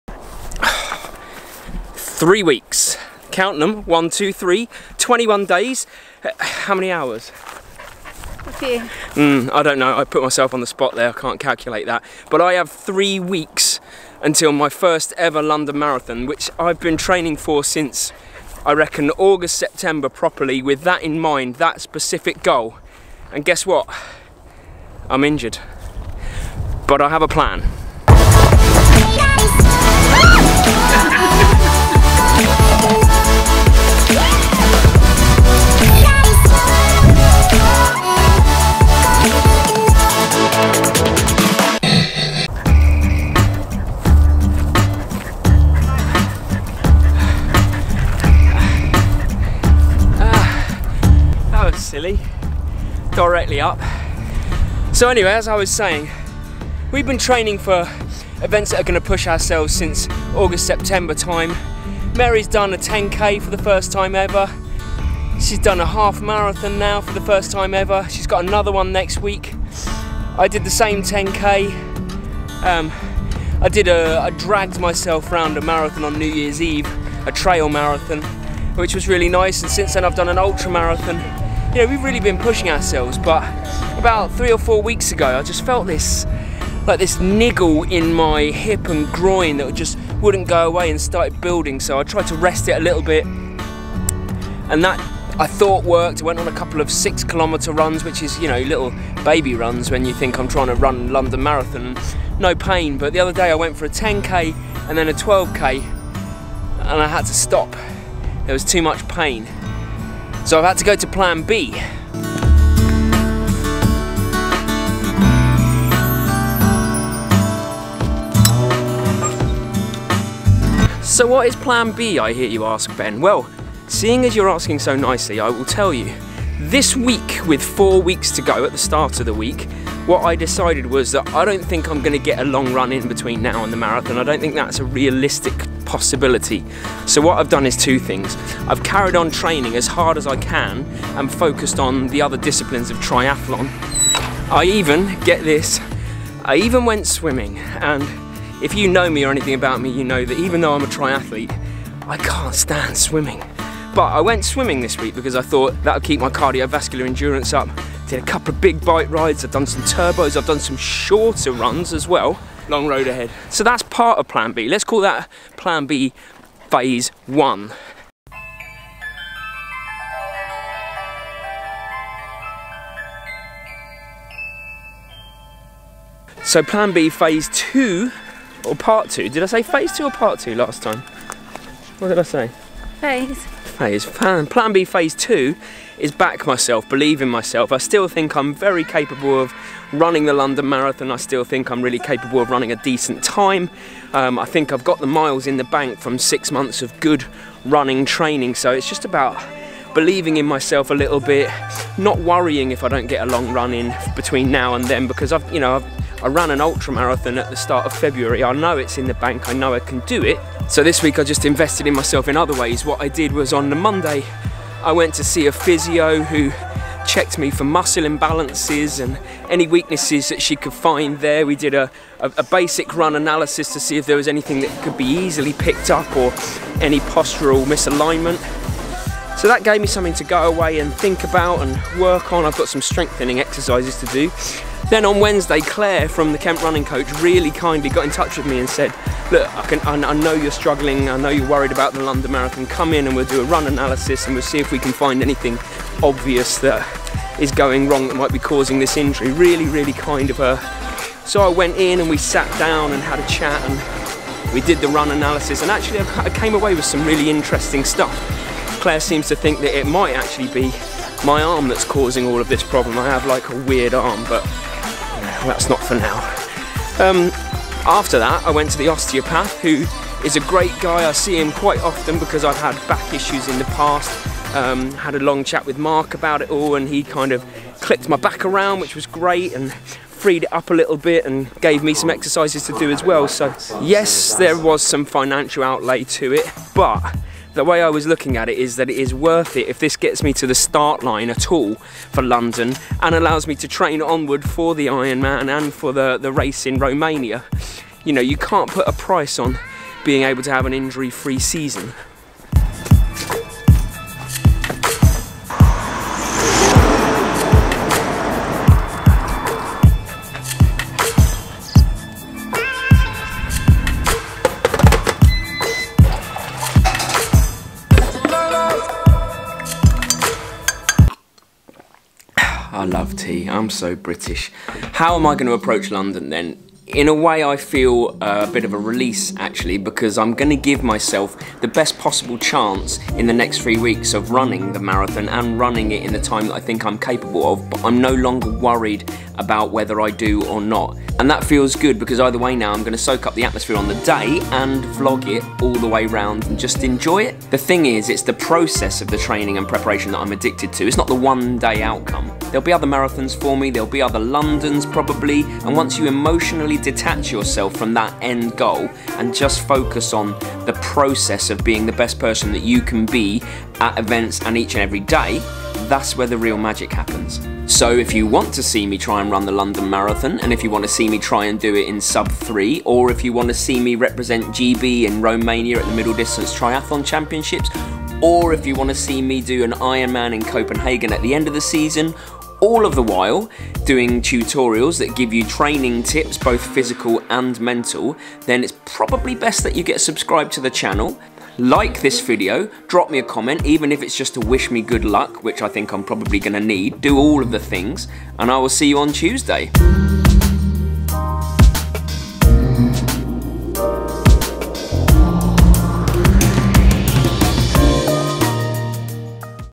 London Marathon Update - 3 Weeks to go and guess what?
I’ve been training for over 6 months for this. The London Marathon is in 3 weeks and right now... I don’t know if I’m going to be able to run it how I want. Make no mistake I will be there, I will complete it, and I will record it all but. there’s a bit of a spanner in the works right now. I’m injured.

In this London Marathon Update I’ll go through how I’m approaching the injury and my mindset going in to the biggest race of my life and my approach to training in general. This is going to be a big 3 weeks!! Wish me luck